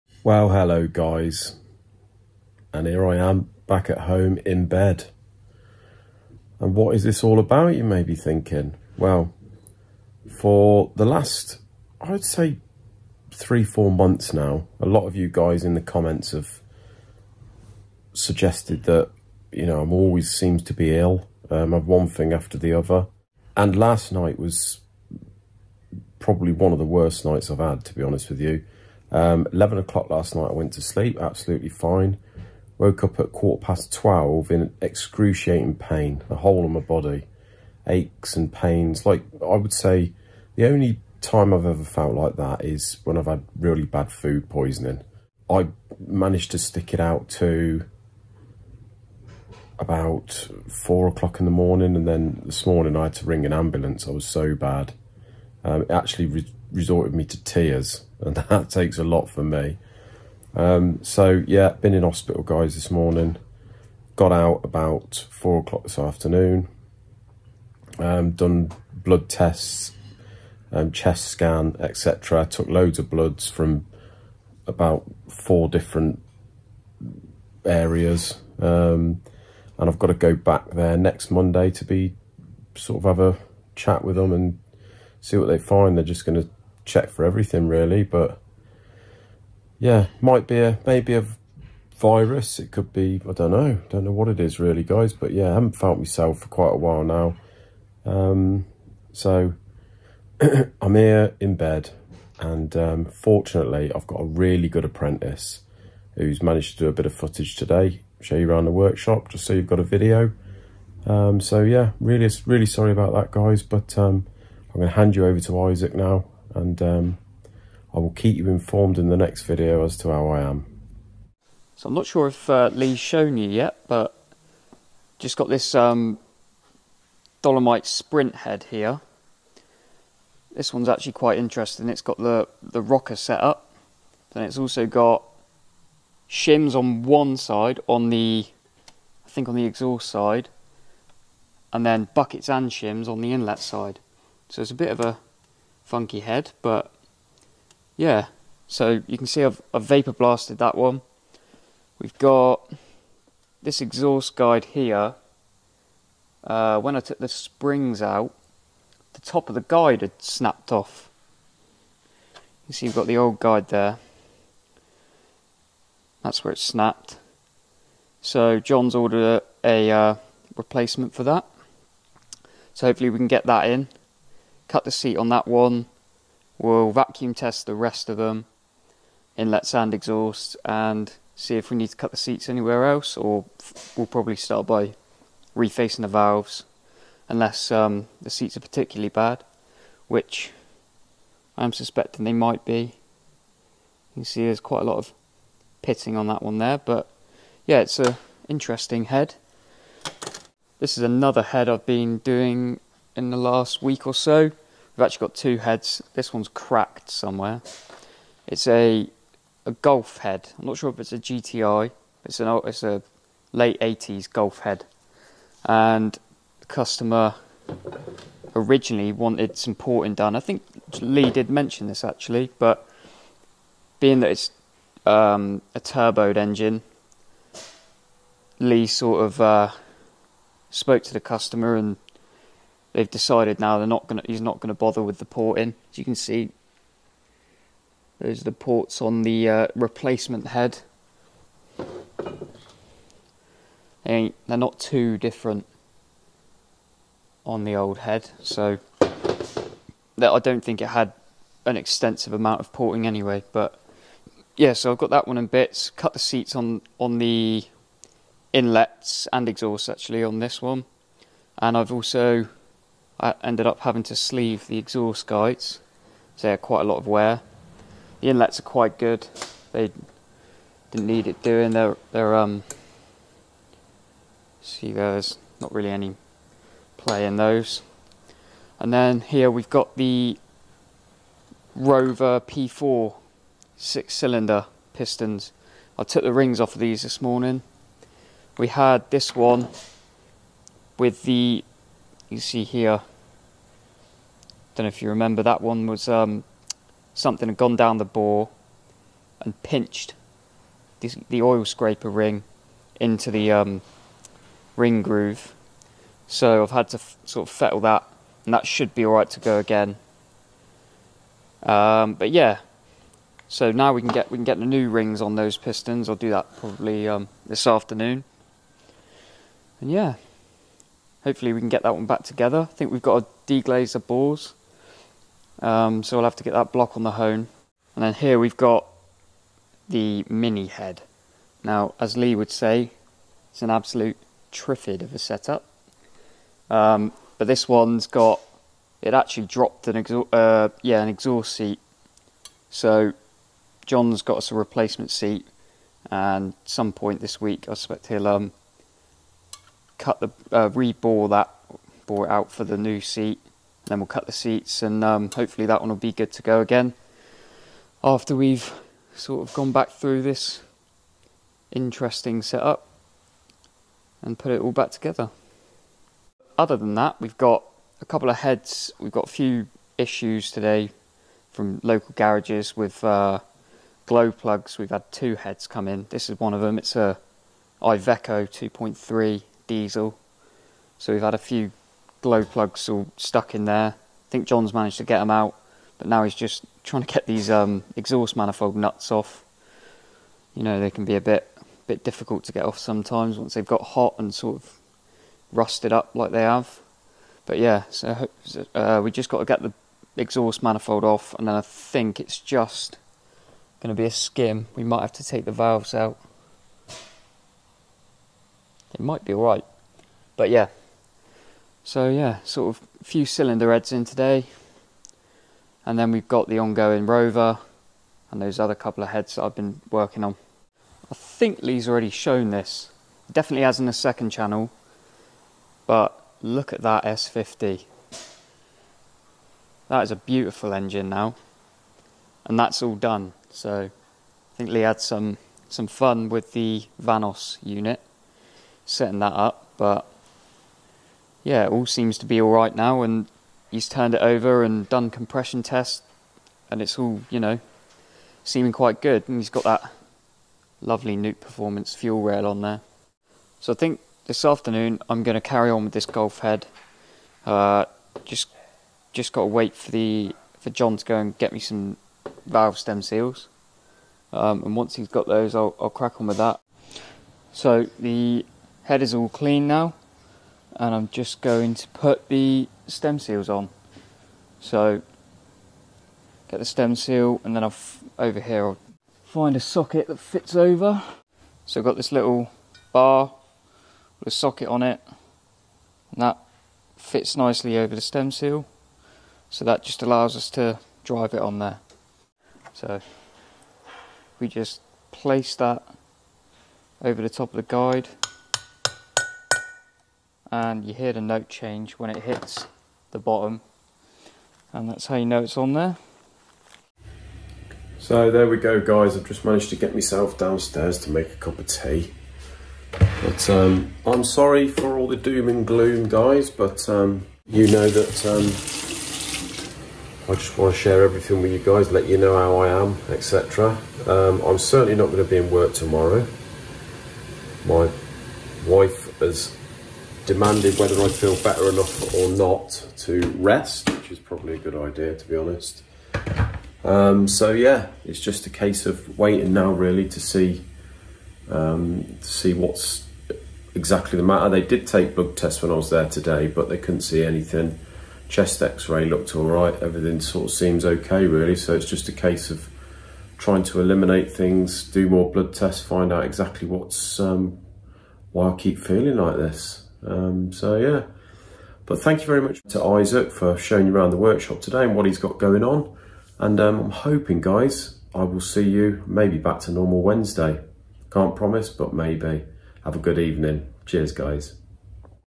Lee gets taken to hospital by ambulance!
Lee gets taken to hospital by ambulance following ongoing illness and Issac saves the day in todays video